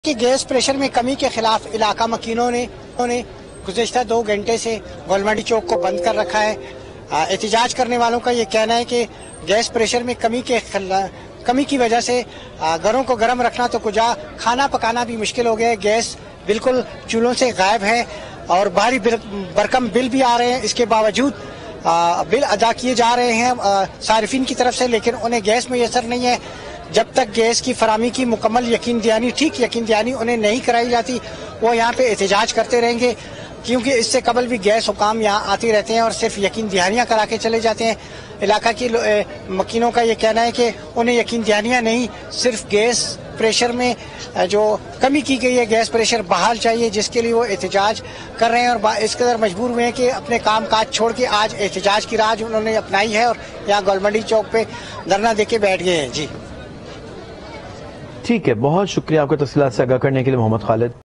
Quetta residents protest against gas loadshedding | Aaj News
Aaj TV Official:
shorturl.at/iBDNX